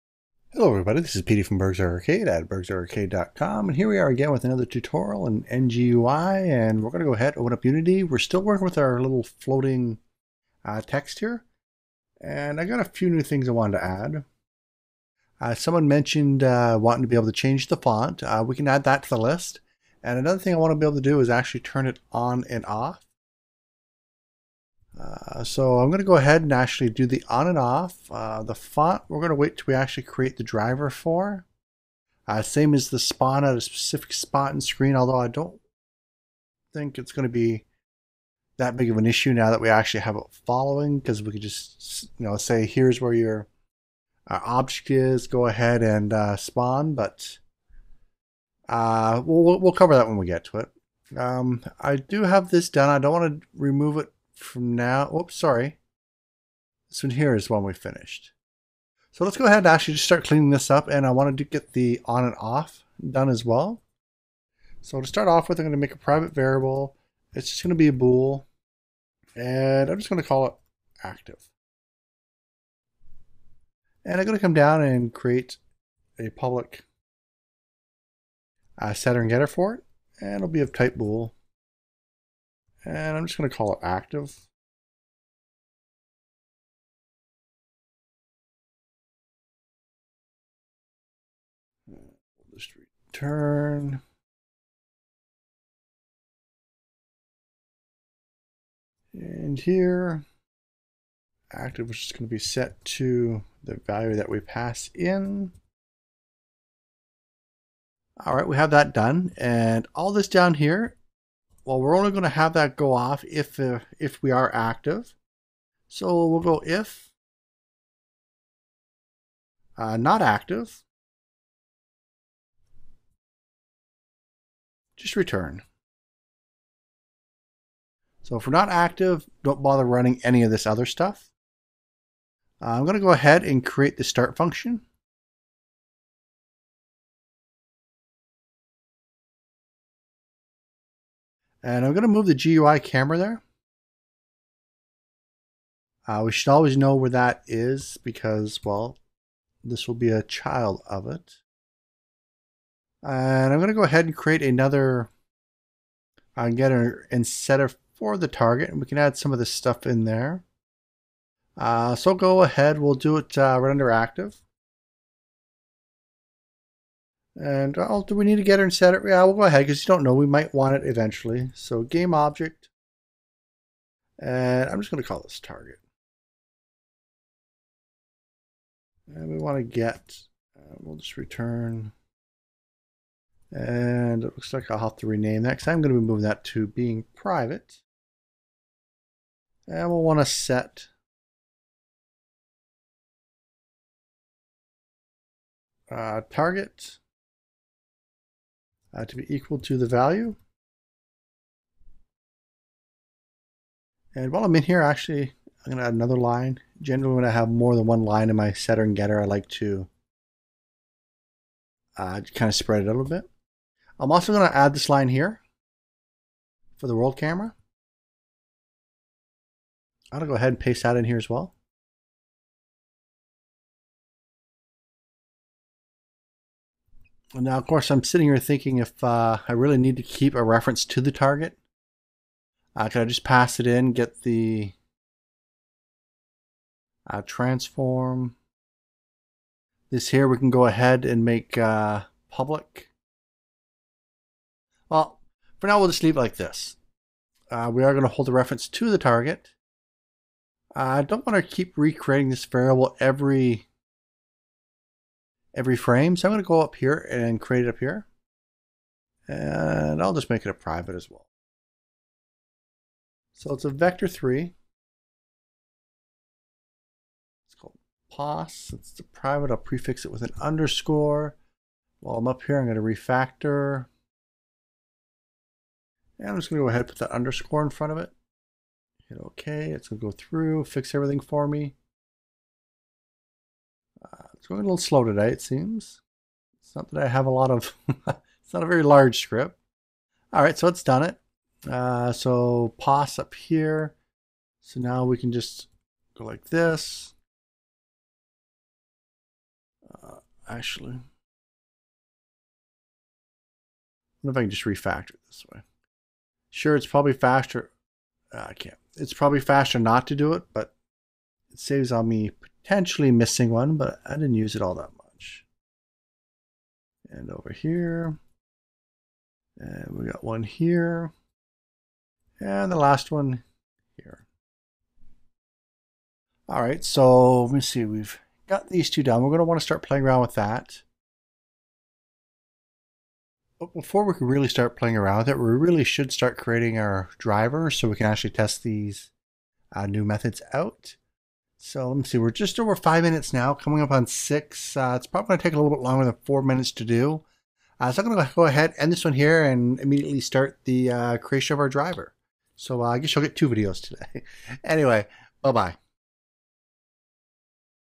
NGUI - Floating Text Clean Up and Activate
IN this installment of our NGUI tutorials, we are going to clean up so of the code from the last tutorial, as well as adding a function to allow us to turn the flaoting text on and off.